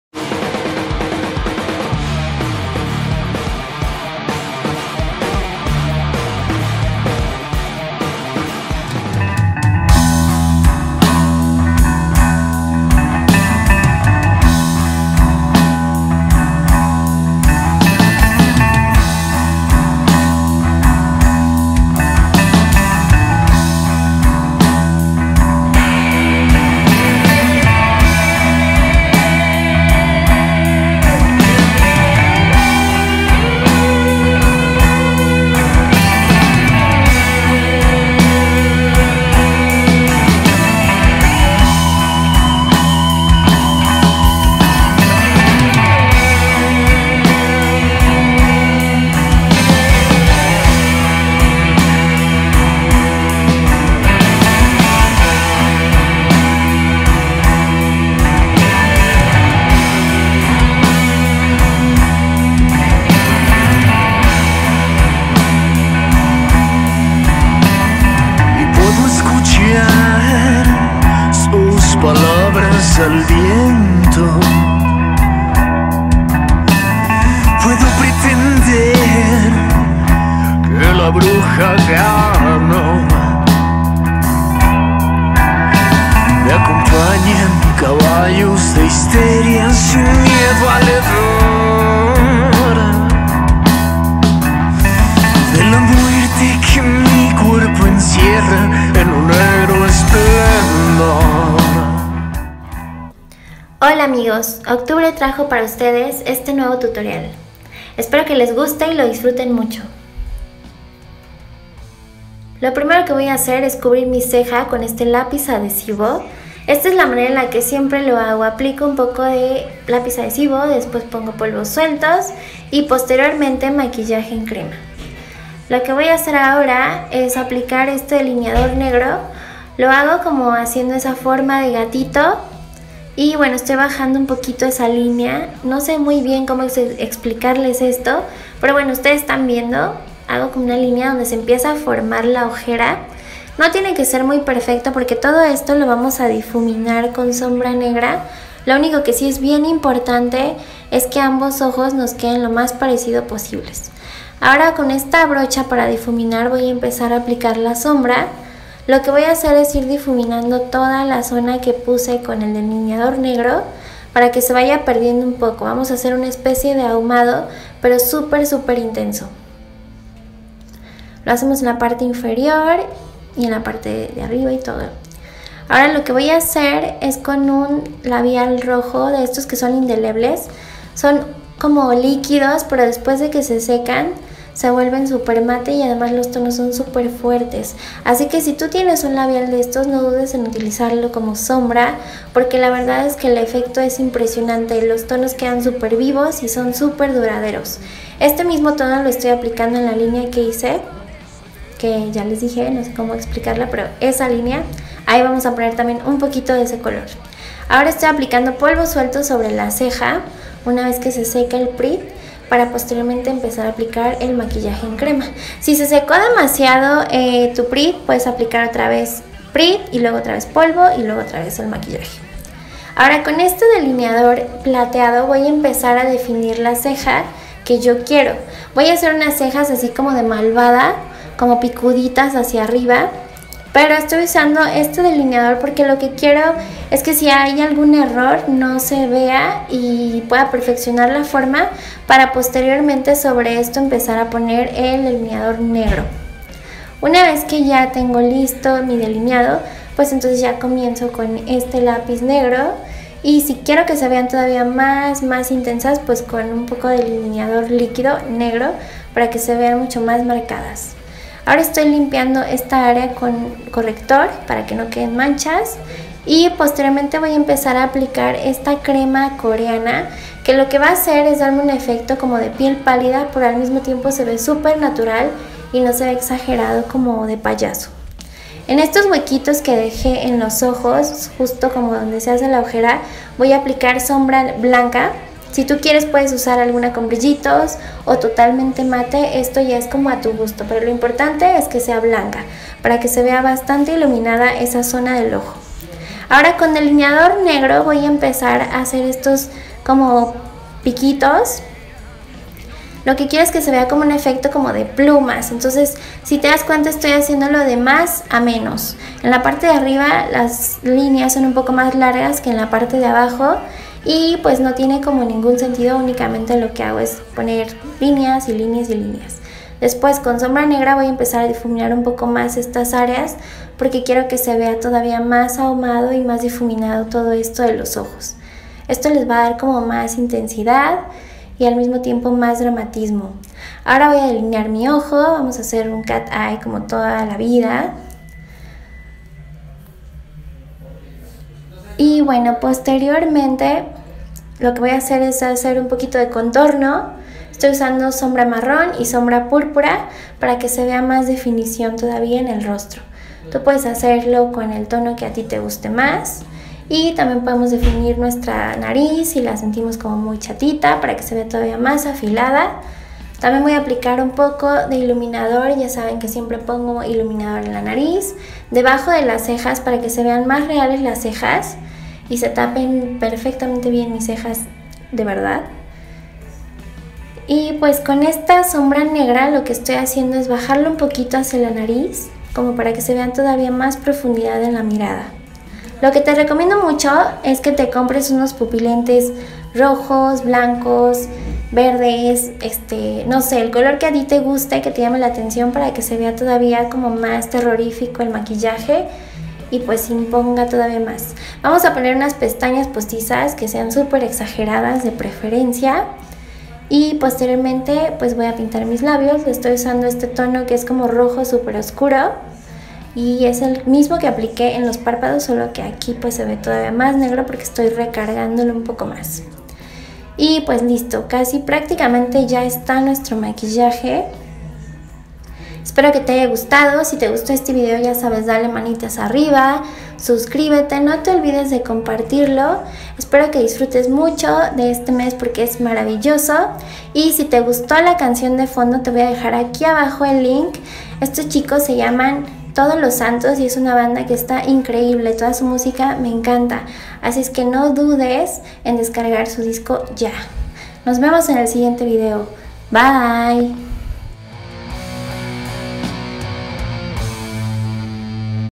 BLACK WITCH MAKEUP- MAQUILLAJE DE BRUJA-LUNAWEILAND
Hola amigos! Estamos a muy pocos días de celebrar la noche de brujas !! Así que no podía evitar hacerles este tutorial ! Espero que les guste y lo pongan en práctica ! Feliz Halloween!!